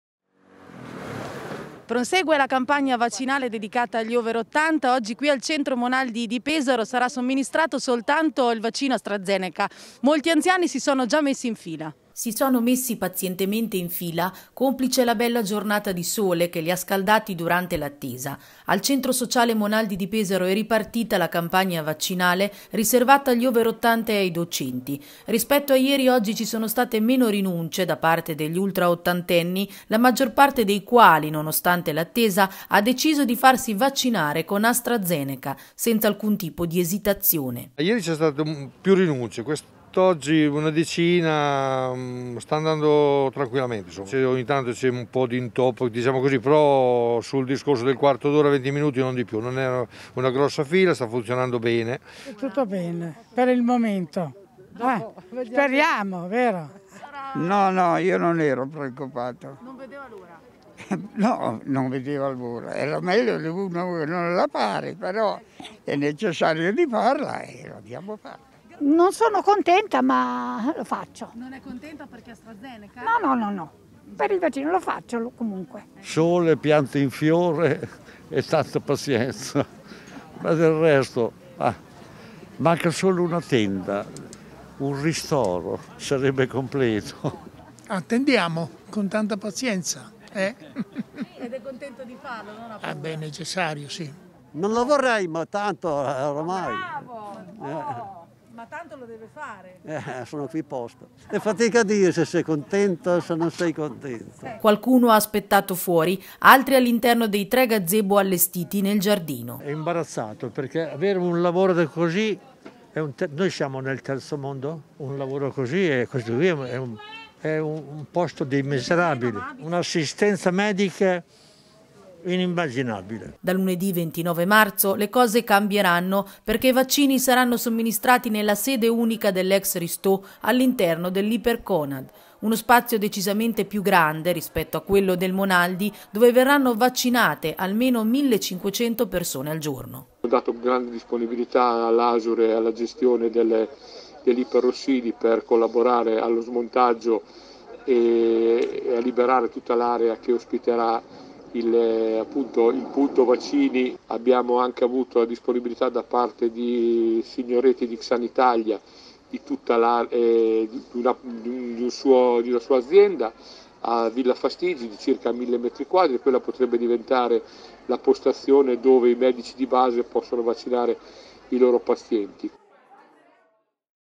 Vaccini over 80: "AstraZeneca? Io lo faccio"
Lunedì al Centro Sociale Monaldi a Pesaro sono proseguite le somministrazioni di AstraZeneca. Meno, rispetto a ieri, gli anziani che lo hanno rifiutato.